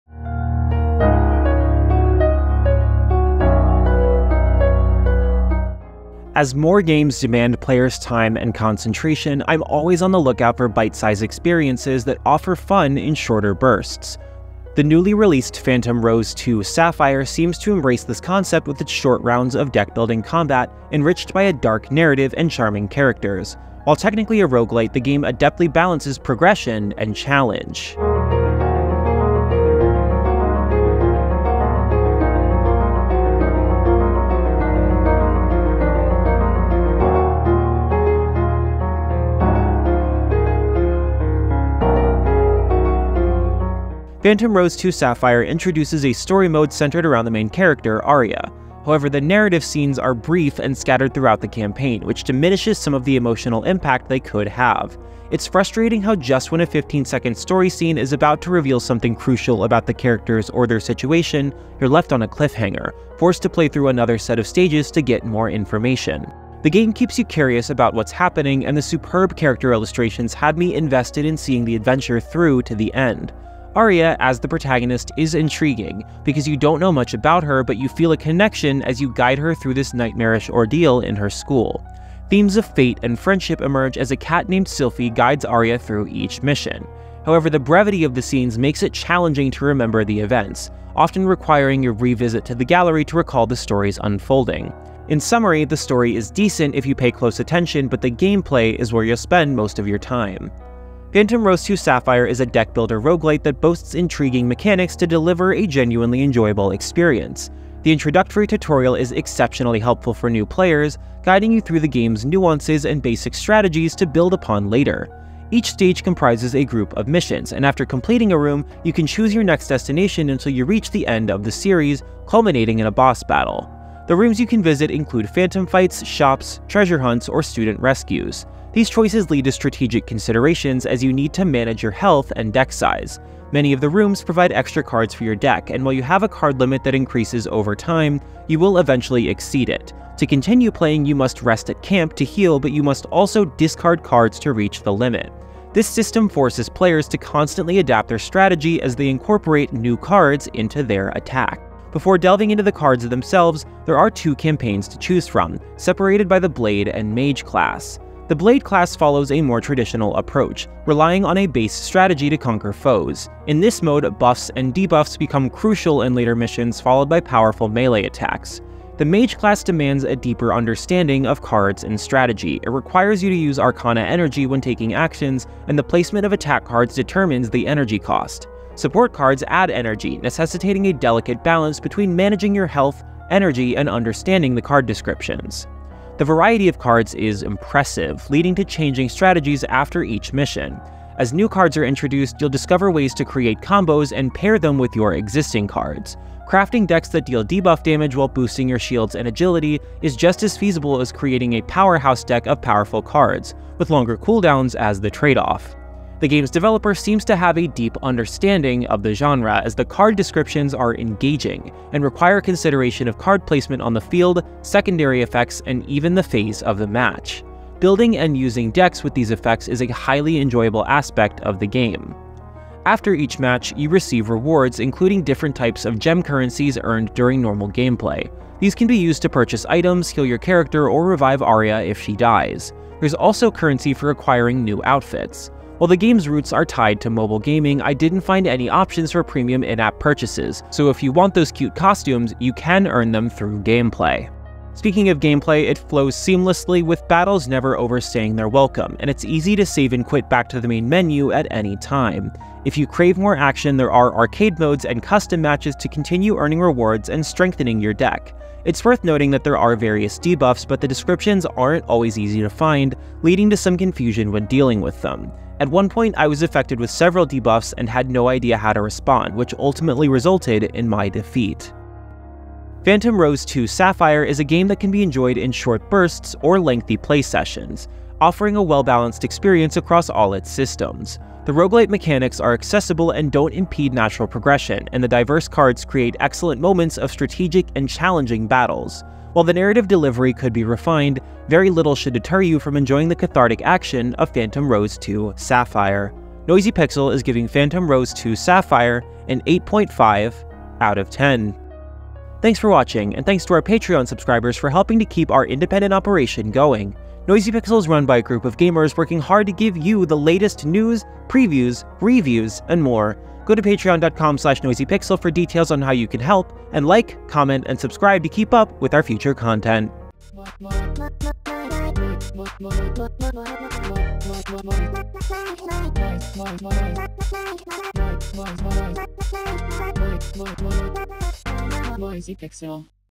Phantom Rose 2 Sapphire Review - Not Your Everyday Waifu Deckbuilder
Phantom Rose 2 Sapphire is a game that can be enjoyed in short bursts or lengthy play sessions, offering a well-balanced experience across all its systems. The roguelite mechanics are accessible and don't impede natural progression, and the diverse cards create excellent moments of strategic and challenging battles. While the narrative delivery could be refined, very little should deter you from enjoying the cathartic action of Phantom Rose 2 Sapphire.